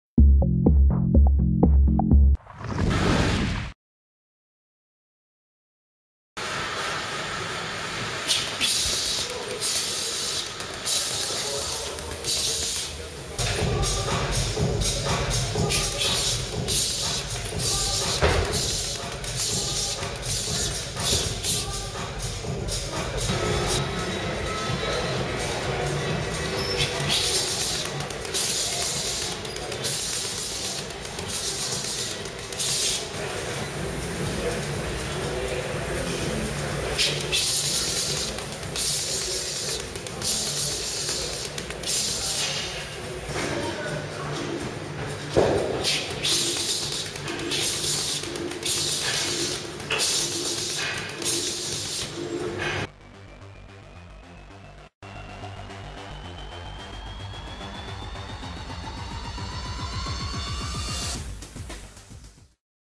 NON TEXTBOOK BOXING PUNCHES uses economy of movement to generate amazing speed and real power
Real punches thrown with genuine power backs up a 25kg bag with10 oz gloves. Extended arms. Straight effective punches. 9-10 punches per sec at best.

This is a demo of the speed and power of the ADS (advanced delivery system) punching technique (Manny pacquiao and other elite boxers uses this exact technique). What you see in this video is a drill and its not meant to be used literally as seen here, not in a boxing match anyway, for the reason it's not wise to throw combos being so upright for too long (incase of a counter). But might be useful if you're put under pressure in a tight spot when you need to blast yourself out of trouble but shouldnt be repeated too often. The technique can be incorporated to throw jabs, hooks, and uppers cuts to improve speed and power shown in my other videos but looks nothing like what you see here, however this is the core technique. This is the real deal, to my knowledge the punches performed here are the only valid fastest punches seen on youtube (9-10 punches per sec) that can back up a 25kg solid maize bag (the weight of a sack of potatoes). Im happy to challenge anyone to repeat my demo. Other fast punches demos I've seen are mostly gimmicks consisting of circular punches (lack real power) or just arm punches and are only fast because they wear wraps not boxing match gloves.